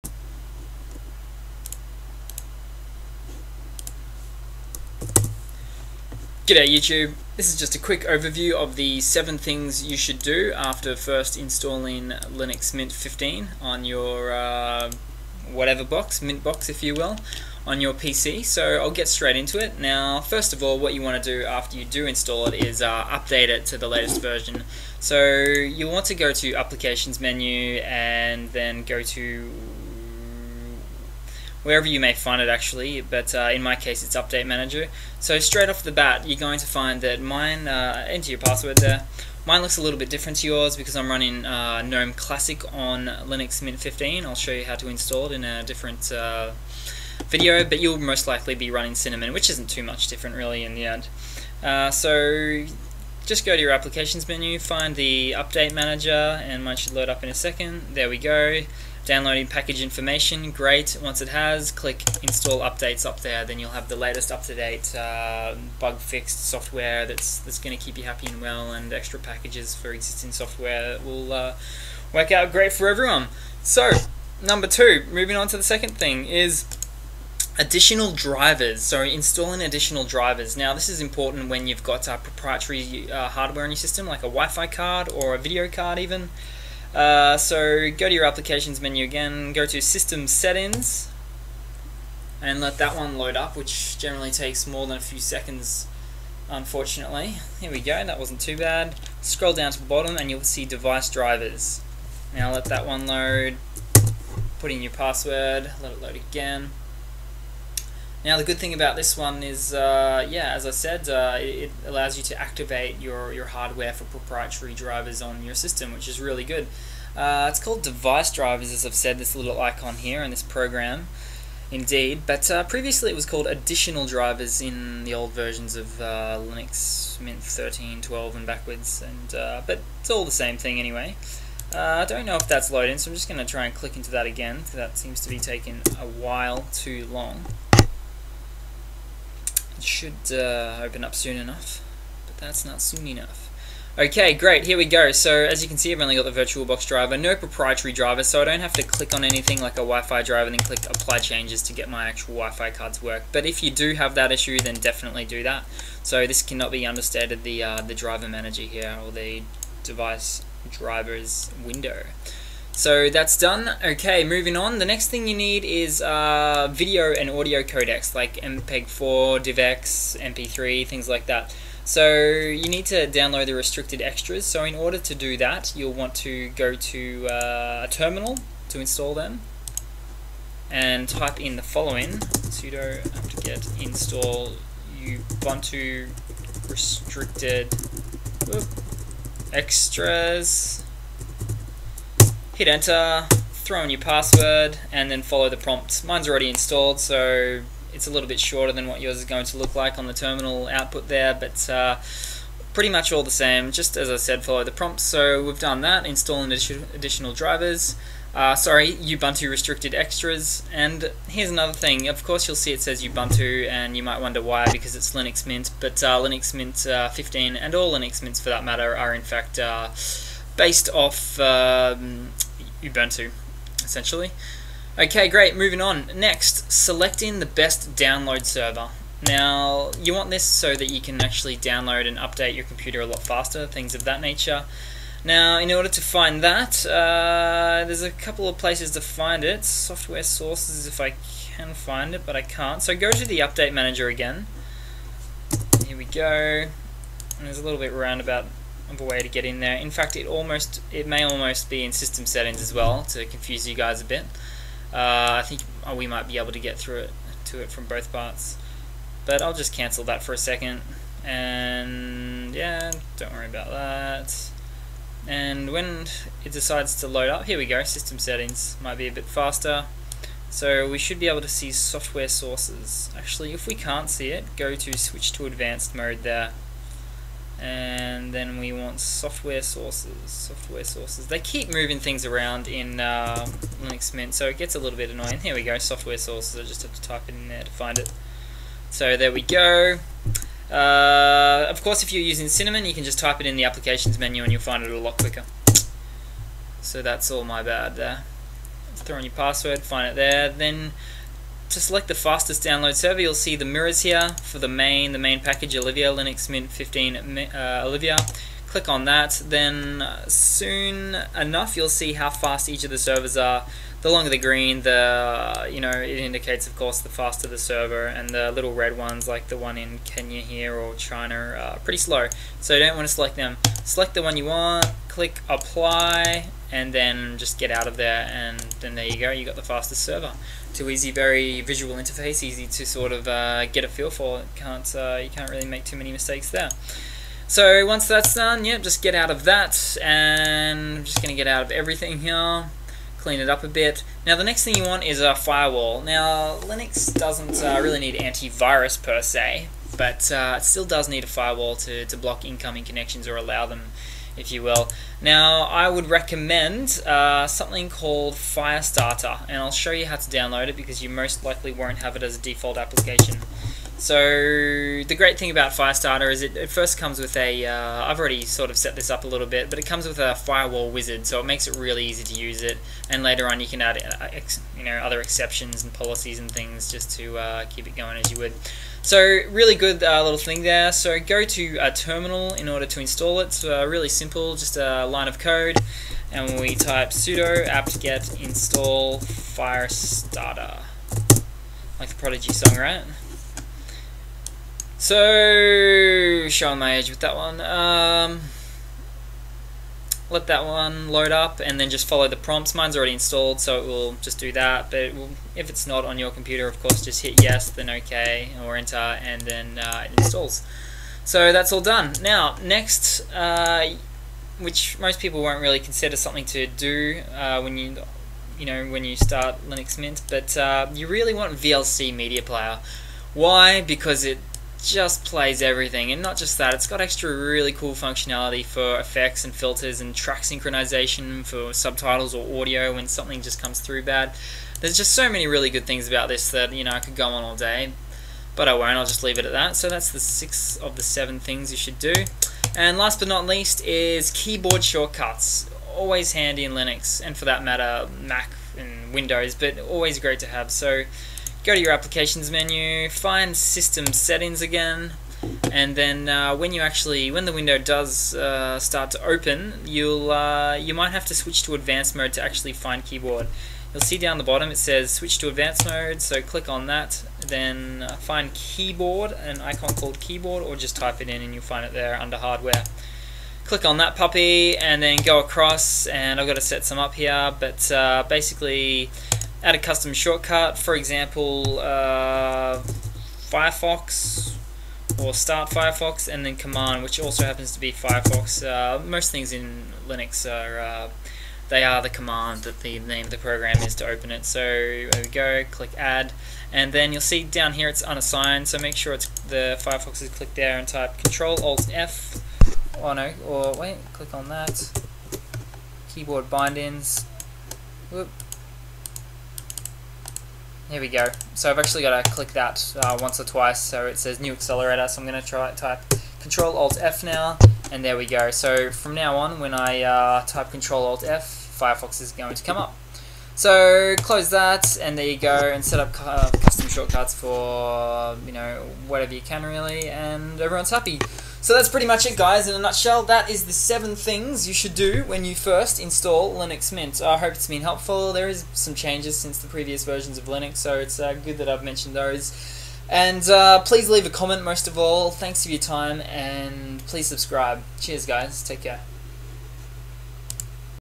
7 Things to do after Installing Linux Mint 15
The undeniable 7 things you'll need to do after first installing Linux Mint 15